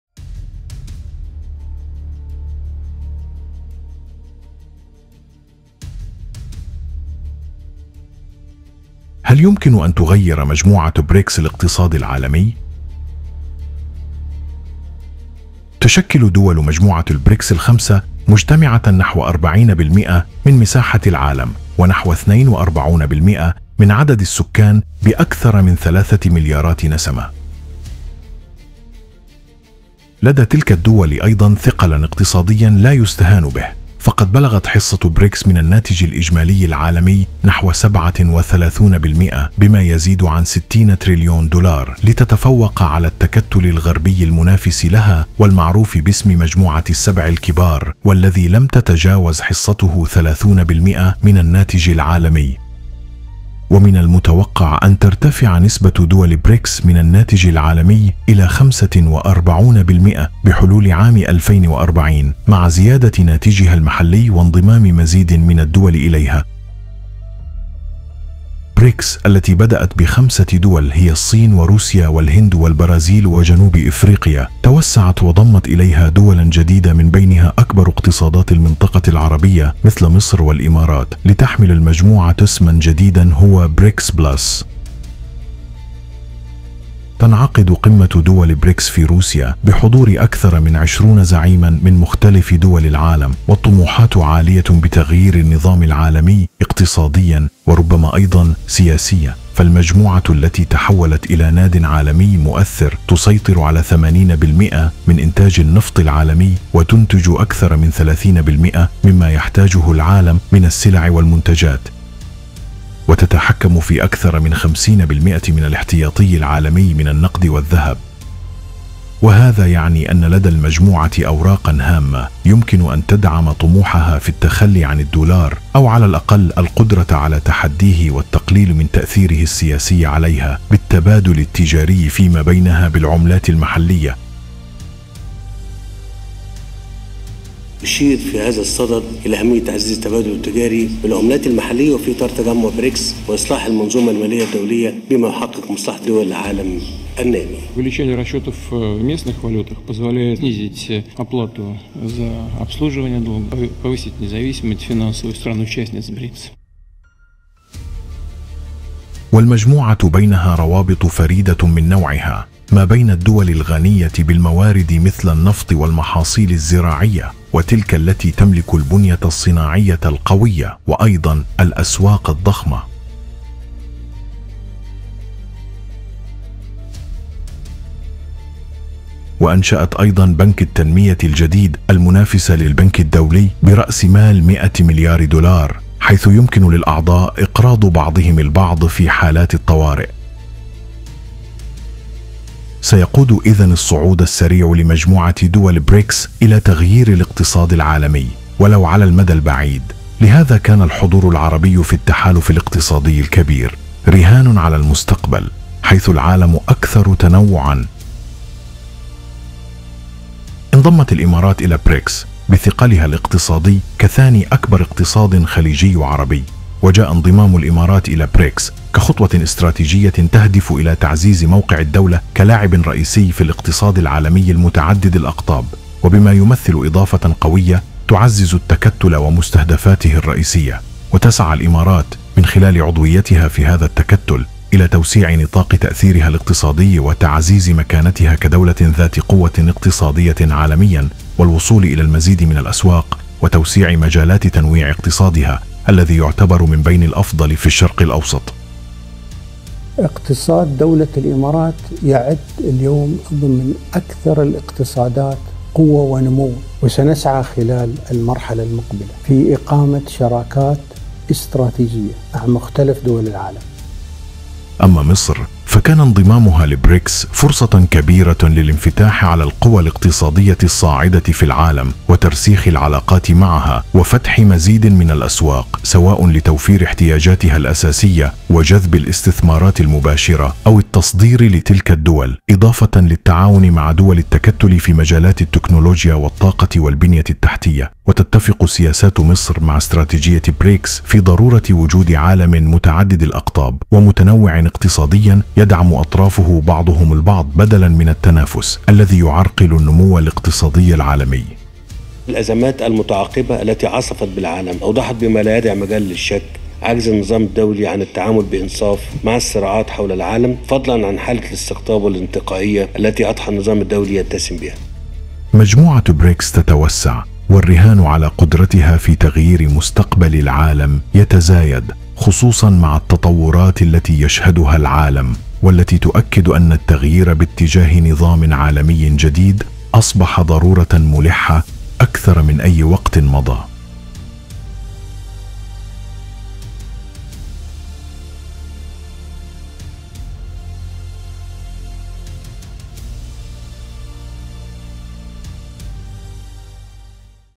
ماعت جروب|بوتين وقادة العرب بمهمة حساسة ببريكس.إنهاء الحرب وبورصة حبوب ومعادن نادرة وقرارعن أوكرانيا
هل يمكن أن تُغيّر مجموعة بريكس الاقتصاد العالمي؟
تنعقد قمة دول بريكس في روسيا، بحضور أكثر من 20 زعيمًا من مختلف دول العالم.
والطموحات عالية بتغيير النظام العالمي، اقتصاديًا وربما أيضًا سياسيًا.
فالمجموعة التي تحولت إلى نادٍ عالمي مؤثر، تسيطر على 80 % من إنتاج النفط العالمي، وتنتج أكثر من 30 % مما يحتاجه العالم من السلع والمنتجات، وتتحكم في أكثر من 50 % من الاحتياطي العالمي من النقد والذهب.
وهذا يعني أن لدى المجموعة أوراقا هامة، يمكن أن تدعم طموحها في التخلي عن الدولار، أو على الأقل، القدرة على تحديه والتقليل من تأثيره السياسي عليها، بالتبادل التجاري فيما بينها بالعملات المحلية.
والمجموعة بينها تكامل فريد من نوعه، إذ تجمع بين الدول الغنية بالموارد مثل النفط والمحاصيل الزراعية، والبنية الصناعية القوية، وأيضا الأسواق الضخمة.